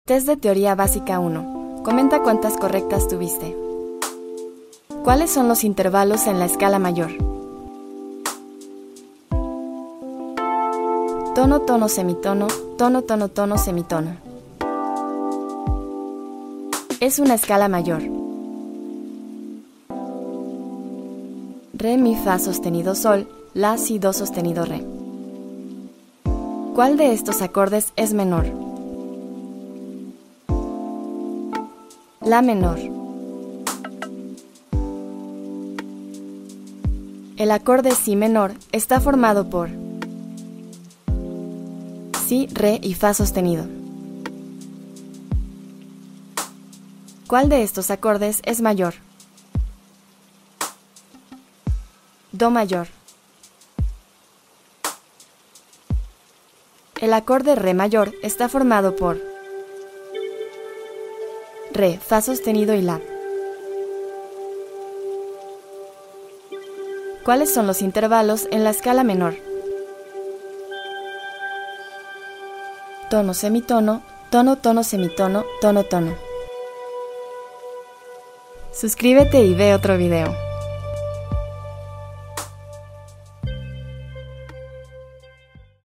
¿Cuánto SABES de TEORÍA MUSICAL básica? - Haz este test sobre escalas y acordes mayores y menores.
Test de teoría musical básica sobre acordes y escalas mayores y menores.